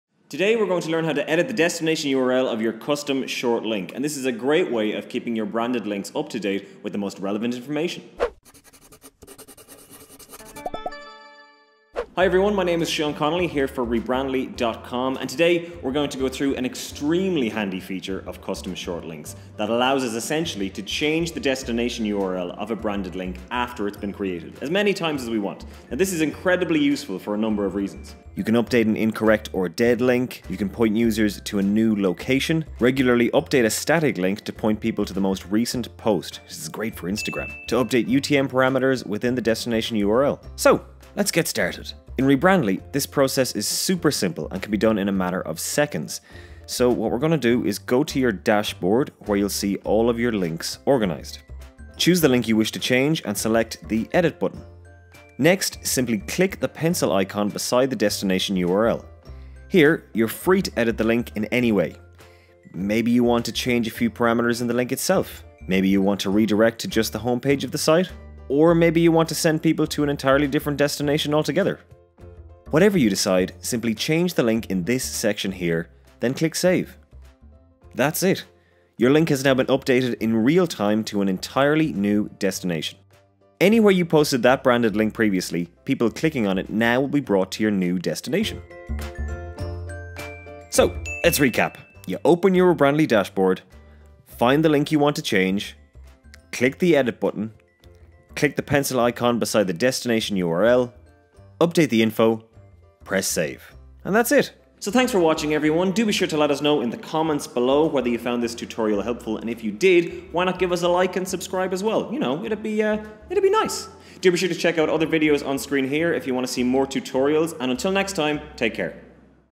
How To Change The Destination URL Of A Custom Short Link
Check out how to quickly change the destination URL of a custom short link - a super handy feature!

Ever share a link on social media and realize the destination URL or landing page is the wrong one, or not working? OH DEAR!

With some URL shorteners you are completely out of luck, but not when you're using Rebrandly! Here's how easy it is to change the destination URL for a custom short link.

To do this you will want to get access to your own free URL shortener with Rebrandly

Rebrandly: Your brand on your links 🔗 Rebrandly is the easiest way to create and share branded short links using a custom domain name!